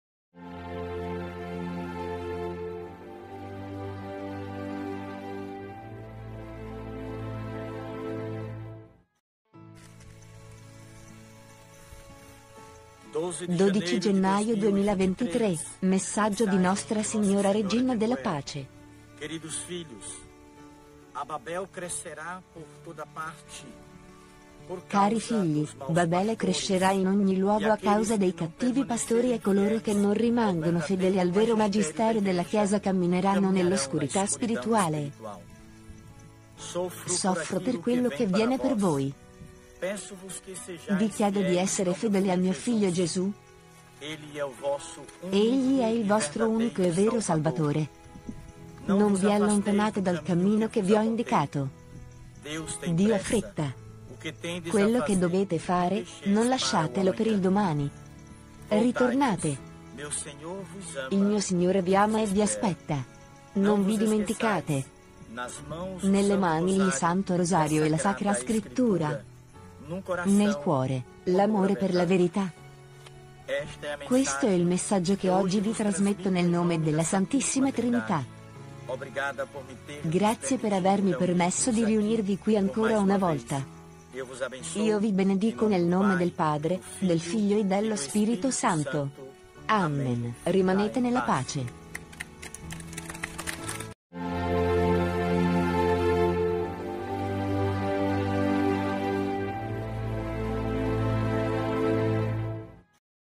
Messaggio di Nostra Signora Regina della Pace, trasmesso il 12/01/2023
Cari figli, Babele crescerà in ogni luogo a causa dei cattivi pastori e coloro che non rimangono fedeli al vero Magistero della Chiesa cammineranno nell'oscurità spirituale. Soffro per quello che viene per voi. Vi chiedo di essere fedeli a Mio Figlio Gesù. Egli è il vostro Unico e Vero Salvatore. Non vi allontanate dal cammino che vi ho indicato. Dio ha fretta. Quello che dovete fare, non lasciatelo per il domani. Ritornate. Il Mio Signore vi ama e vi aspetta. Non vi dimenticate: nelle mani il Santo Rosario e la Sacra Scrittura; nel cuore, l'amore per la verità. Questo è il messaggio che oggi vi trasmetto nel nome della Santissima Trinità. Grazie per averMi permesso di riunirvi qui ancora una volta. Io vi benedico nel nome del Padre, del Figlio e dello Spirito Santo. Amen. Rimanete nella pace.

5.388 – Messaggio di Nostra Signora Regina della Pace, trasmesso il 12/01/2023